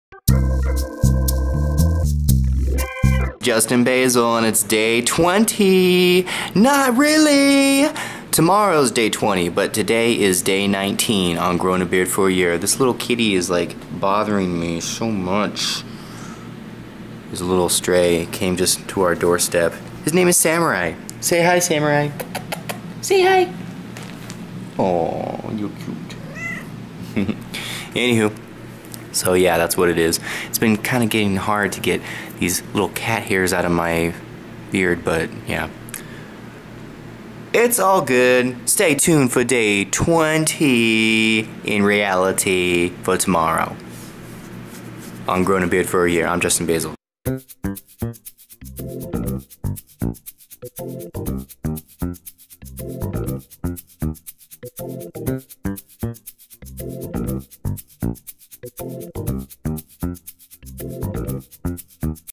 Growing a Beard for a Year - Day19
Starting June 22, 2014, I am going to grow my beard for a whole year. After the year is complete, I'm going to compete in the 3rd Annual Beard and Mustache Competition in Los Angeles, California, on June 22, 2015. After competing, I am donating my beard hair to a charity called, "Locks of Love".

"Care for your hair."

Love,
Justin Basl